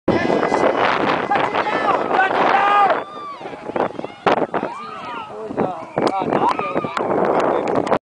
Jayme Reneau
Jayme disobeys coache's order to kick and scores off a long run..good choice Jayme  grins  coach